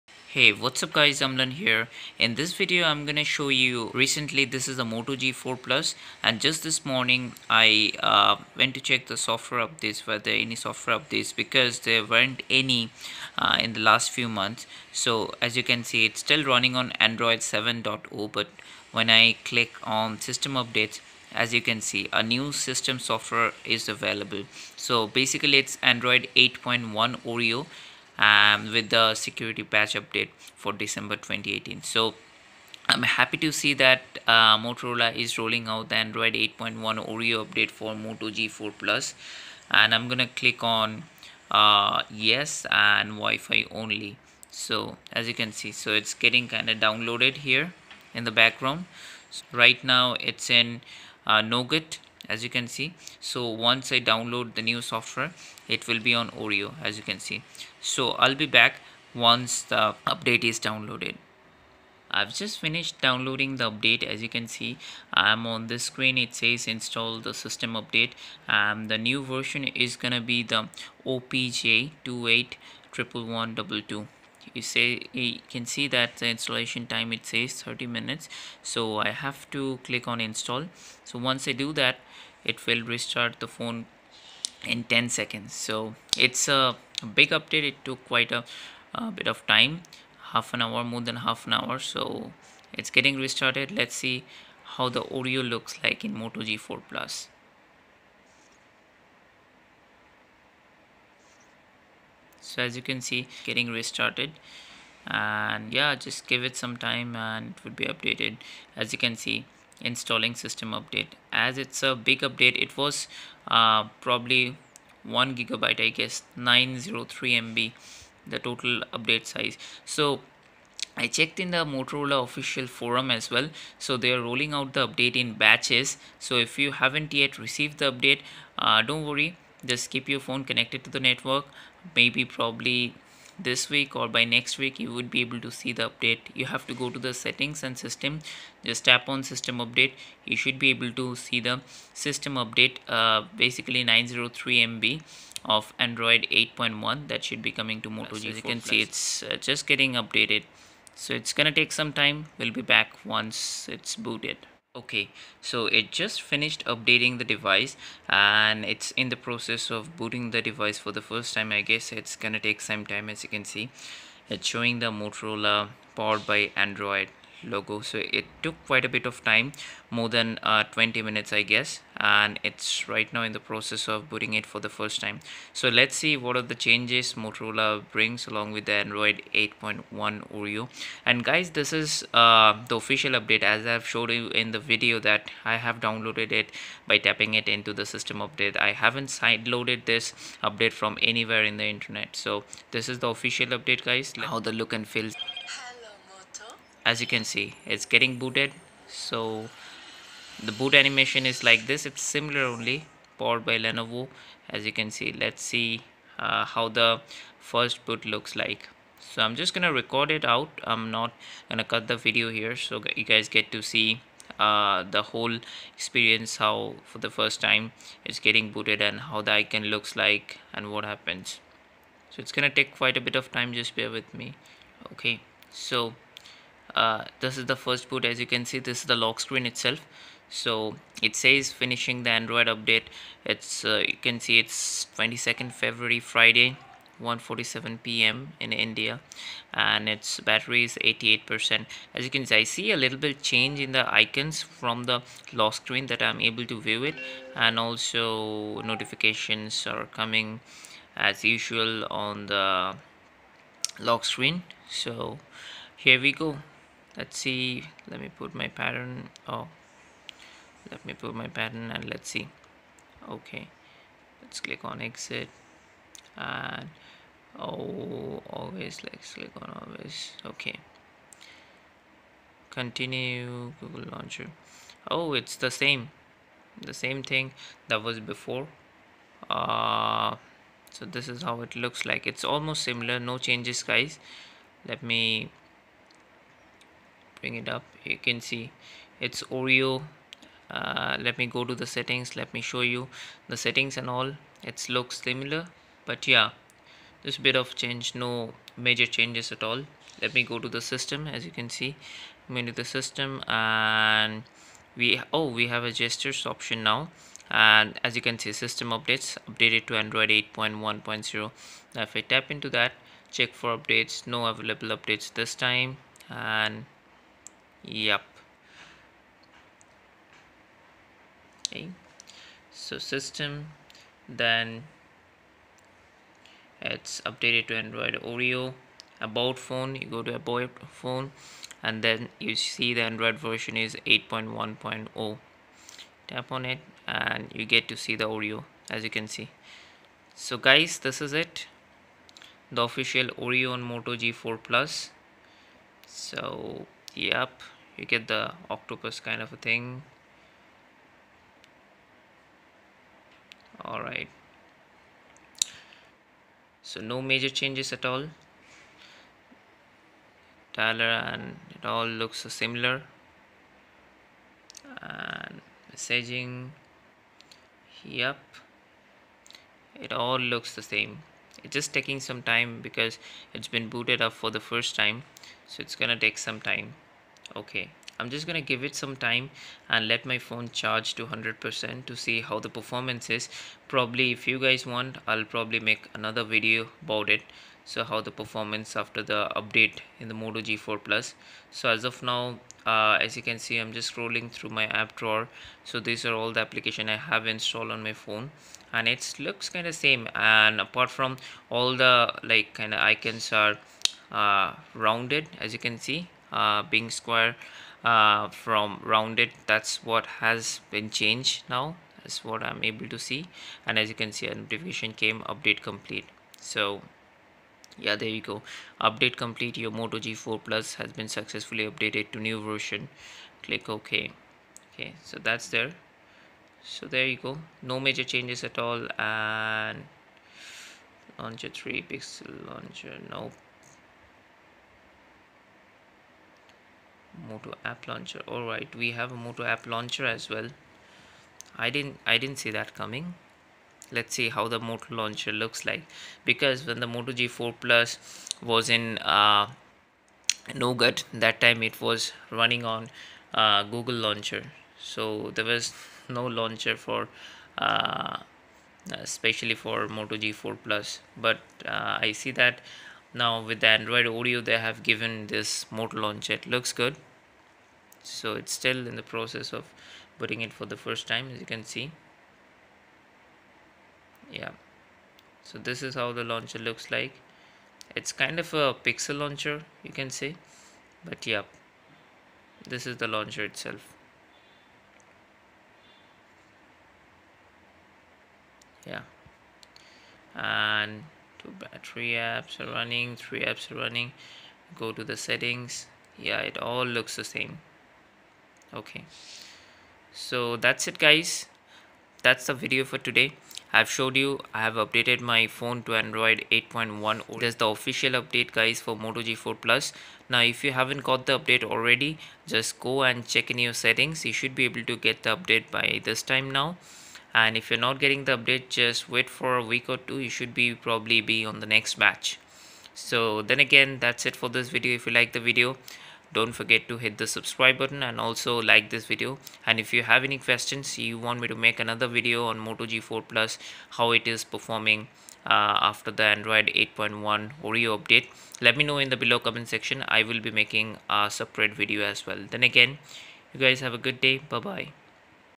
Official 8.1 Oreo Update on Moto G4 plus !
Hi Guys !
I have just got the official Android 8.1 Oreo update on my Moto G4 Plus. In this video I have showed the update and changes after the update of 8.1 on Moto G4 Plus.